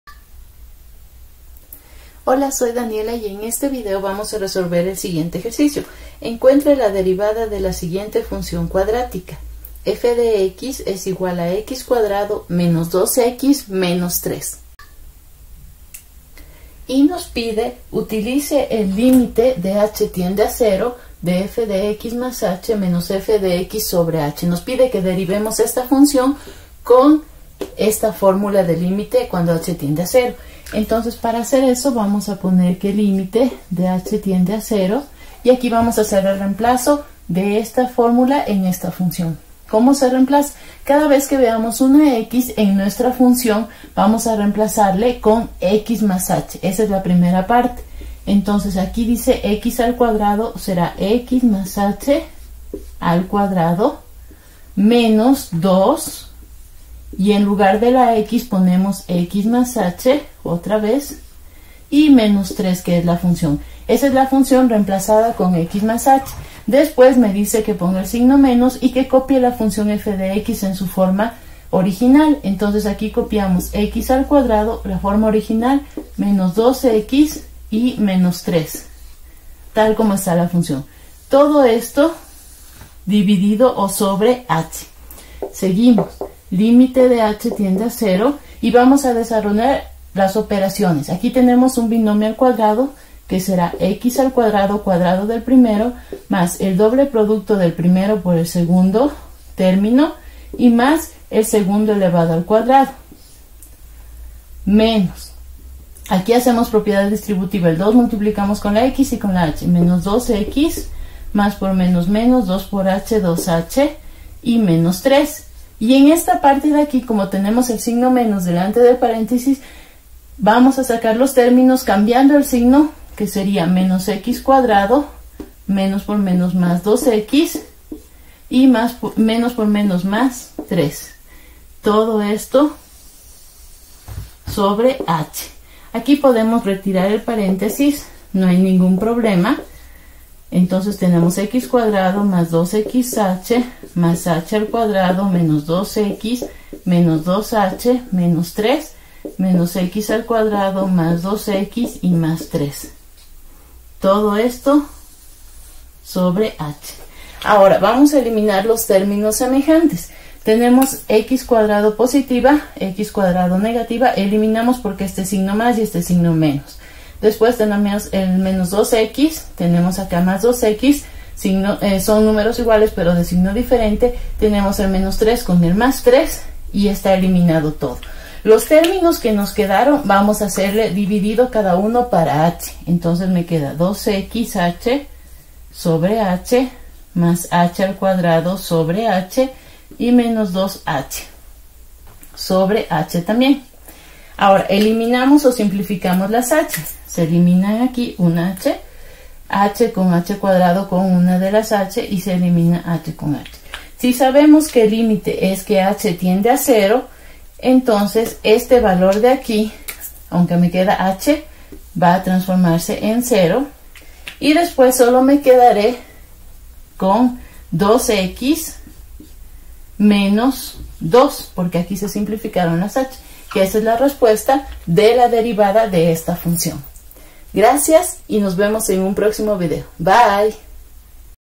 ENCUENTRE LA DERIVADA DE LA SIGUIENTE FUNCIÓN CUADRÁTICA f(x) = x^2 - 2x - 3.
Como derivar una función cuadrática a partir de la fórmula de límite h - 0.